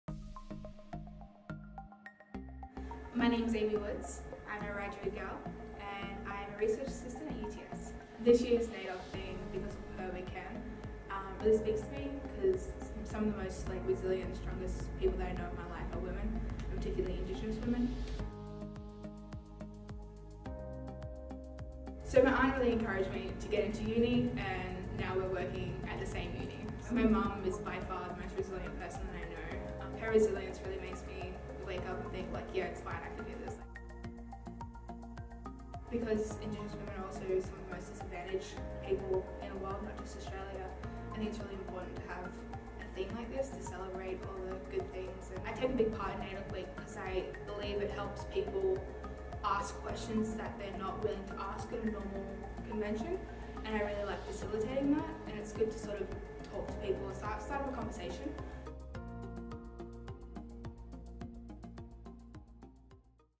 Meet Aimee Woods
Wiradjuri girl and research assistant at UTS on the inspiring Indigenous women who've influenced her life.